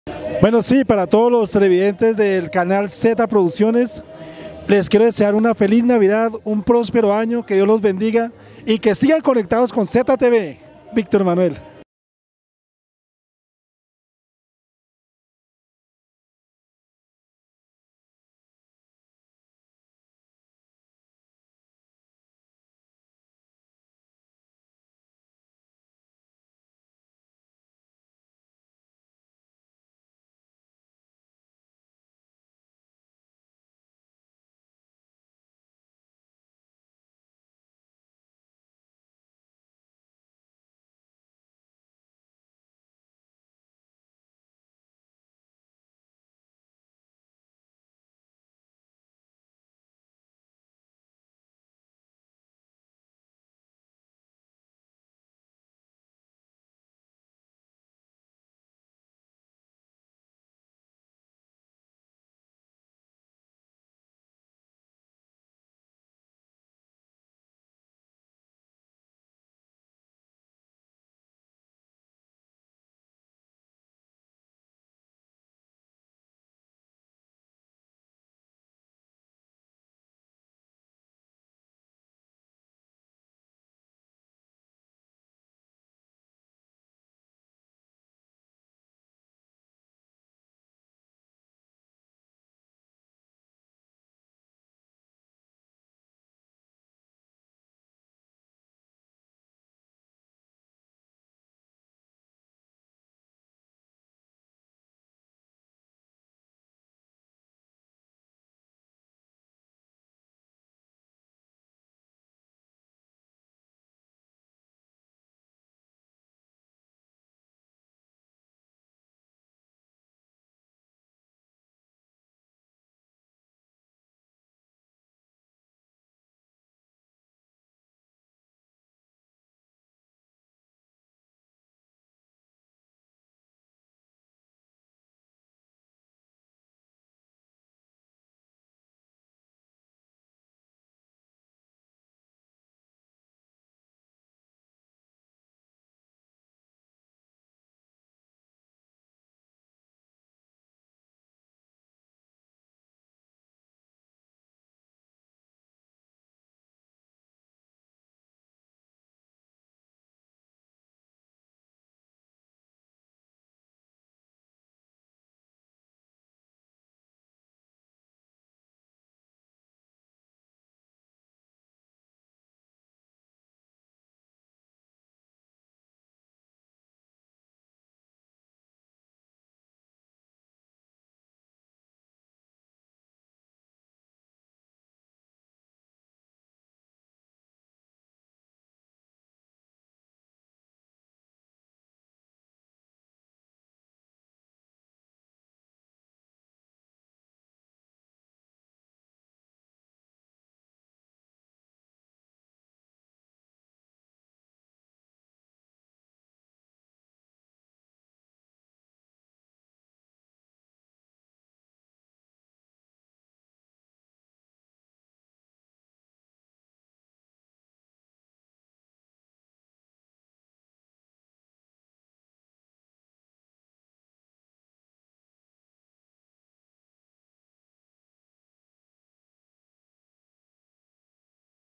Victor Manuel Ampudia Saludo de navidad
El cantante Tolimense nos da un especial saludo de navidad y año nuevo, gracias, y mucos éxitos.